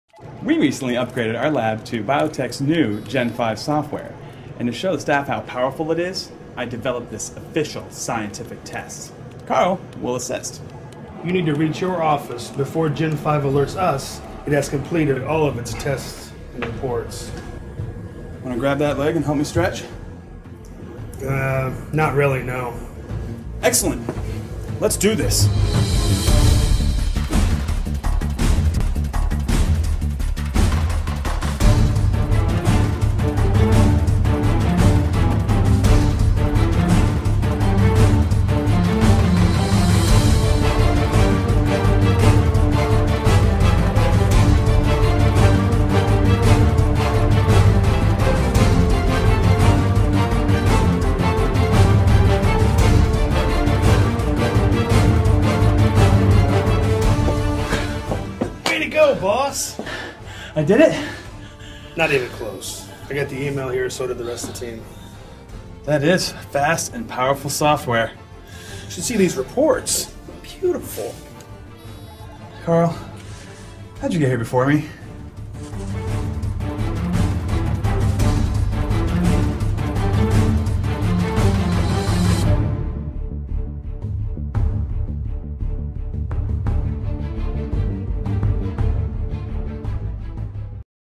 Software Gen5 Biotek
Software para programação e controle da leitora, e redução e análise dos dados obtidos. Possui as seguintes características:

FLx800 (todos os modelos; já incluso em alguns modelos);
PowerWave (todos os modelos; já incluso);
Epoch (já incluso);
ELx808 (já incluso no modelo EL808IU-PC).;
ELx800.

OBS: Pode-se conectar ao computador até duas leitoras simultaneamente (controlando uma por vez).